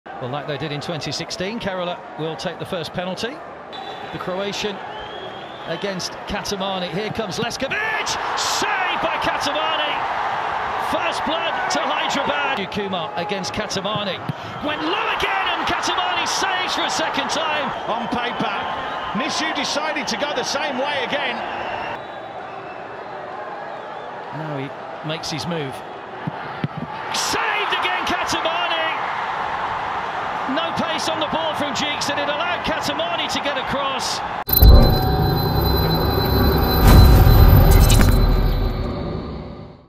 Laxmikant Kattimani - Hero of the Match | HFC (3) 1 -1 (1) KBFC | Hero ISL 2021-22 Final
Kattimani is the Hero of the Match for @hydfcofficial  after saving 3️⃣ penalties during the penalty shootout! 🧤🏆